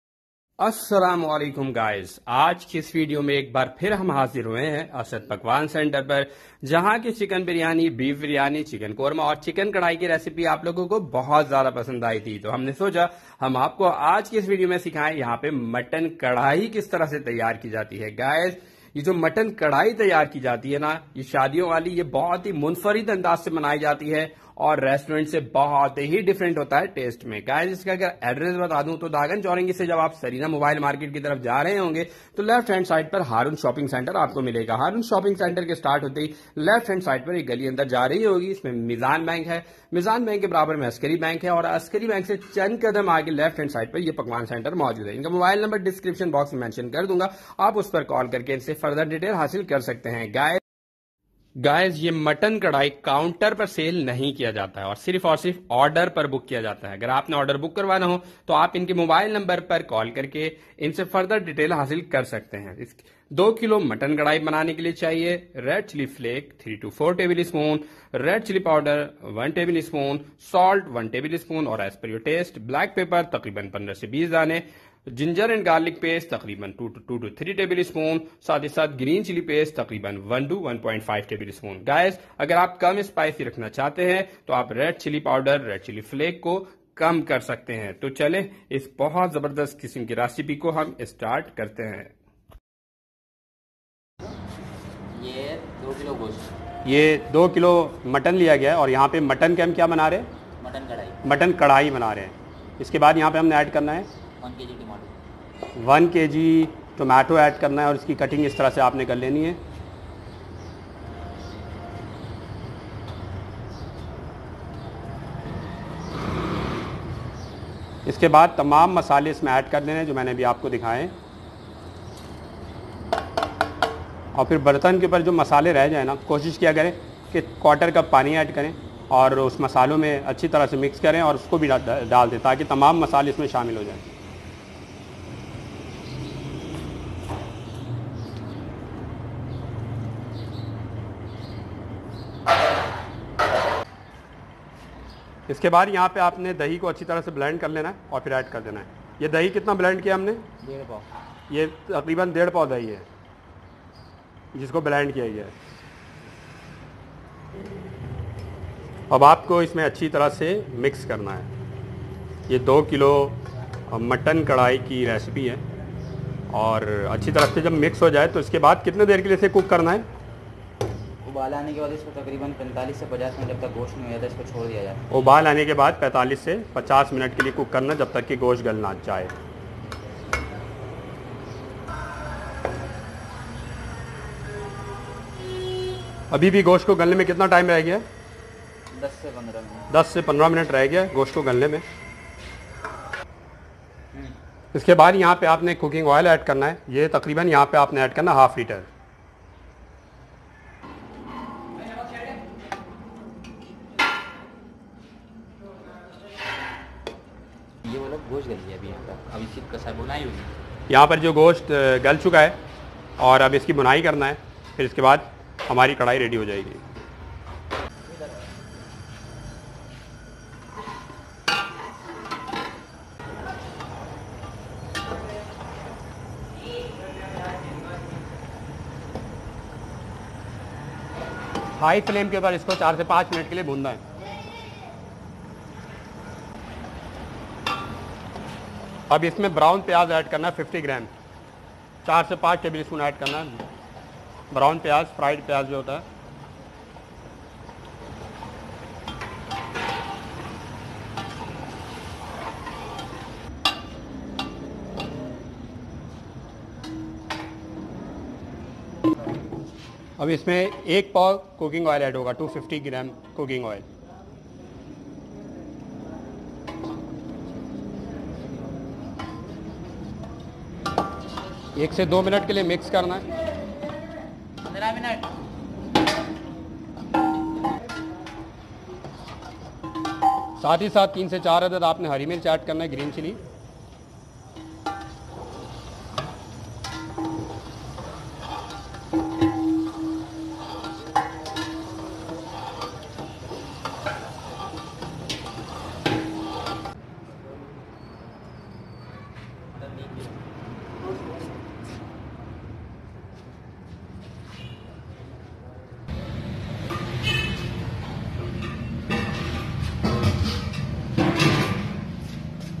Shadiyon Wala Mutton Karahi Recipe By Cooking with Kawish
Today I am going to share shadiyon wali mutton karahi recipe
Address: Shop # R-900/15-A2 Bufferzone Near Haroon Shopping Center north Nazimabad Karachi.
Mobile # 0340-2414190